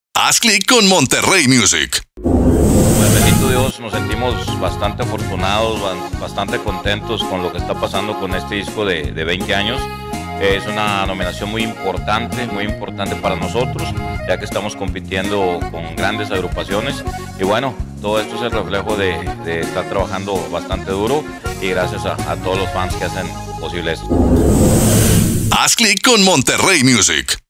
EL PODER DEL NORTE habla sobre su nominación al GRAMMY LATINO
En entrevista, Martín Buenrostro da su opinión sobre la nominación que recibió EL PODER DEL NORTE al Grammy Latino como MEJOR GRUPO NORTEÑO.